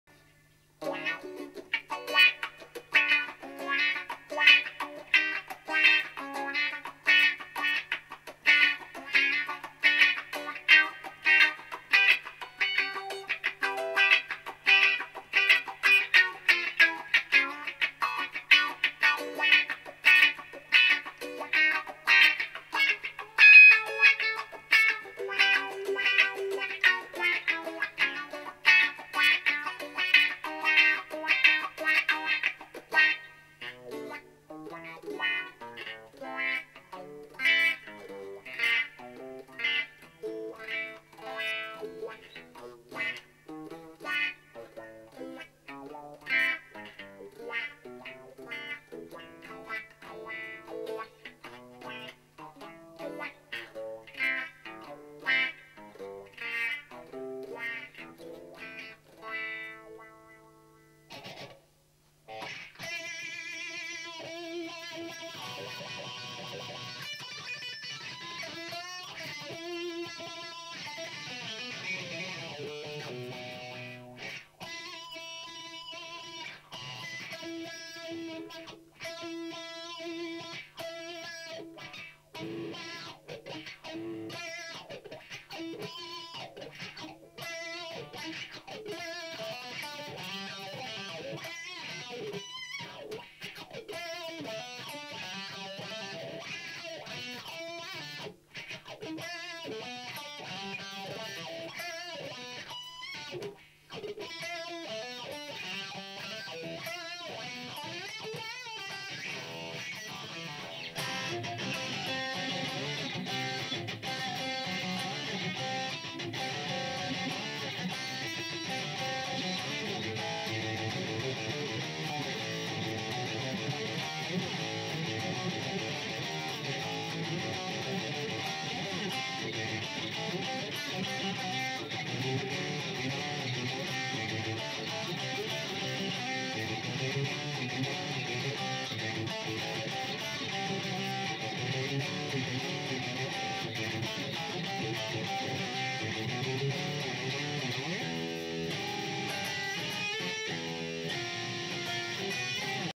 Guitar Funky Jazz - Paul McMahon
Some example guitar playing for an audition for a film to be a guitar player in a 1969 CND rally sat behind the action playing away in the back of a VW Van.